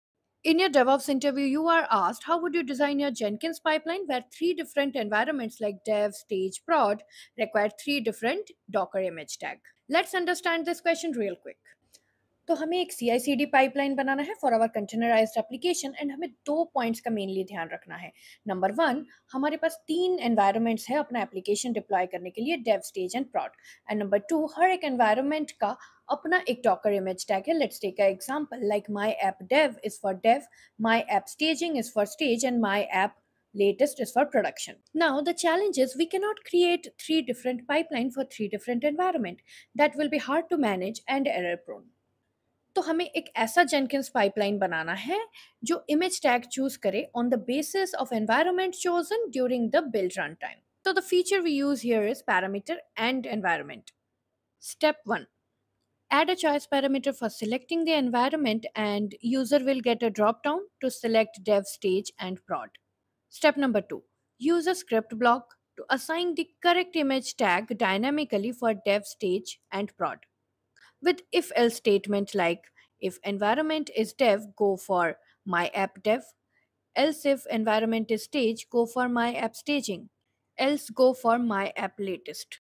One Jenkinsfile for All Envs? Auto-Switch Docker Tags!
Want to deploy your app to Dev, Staging, and Production — all using a single Jenkinsfile? 🤯
In this short, you'll learn how to use Jenkins parameters and Git branch logic to dynamically tag Docker images like:
myapp:dev | myapp:staging | myapp:latest

📌 Topics Covered: ✅ Jenkins Git branch parameter
✅ Dynamic Docker tagging
✅ Real-world CI/CD pipeline logic
✅ DevOps interview tips
✅ English + Hindi explanation

🔧 Tools Used: Jenkins, Docker
🎯 Skill Level: Beginner to Intermediate

👉 Perfect for DevOps learners, interview prep, and daily CI/CD tasks.